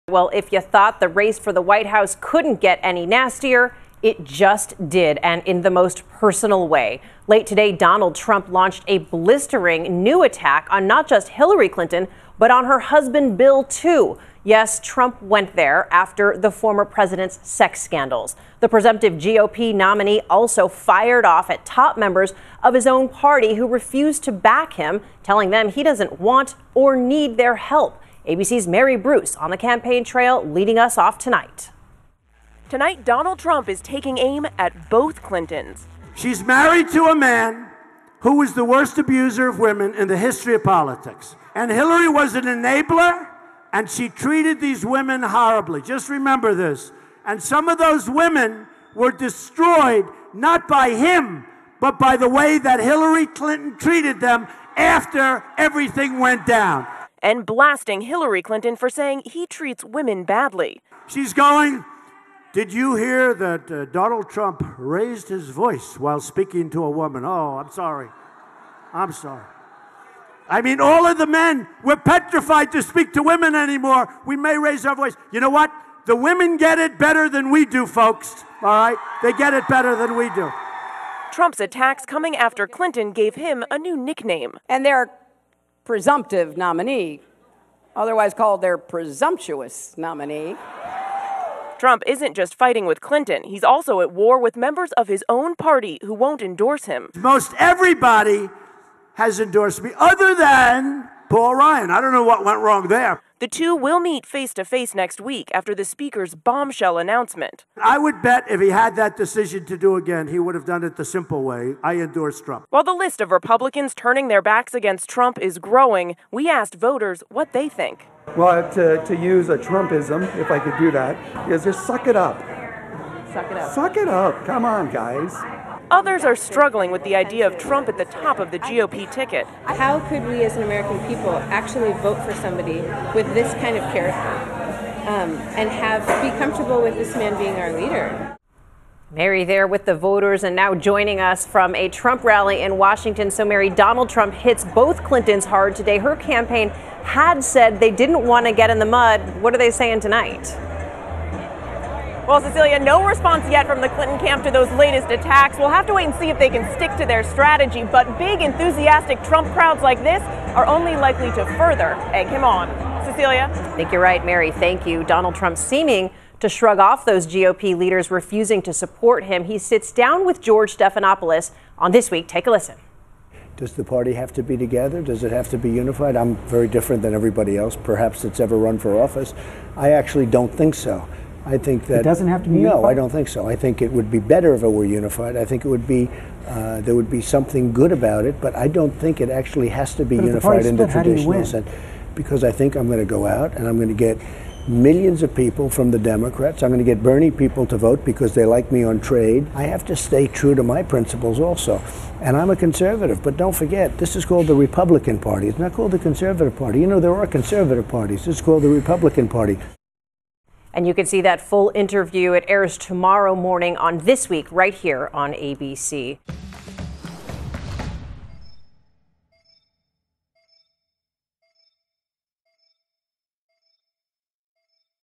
Donald Trump Fires Away at Both Bill and Hillary Clinton
The Republican frontrunner calls former president an abuser of women and Hillary Clinton an "enabler."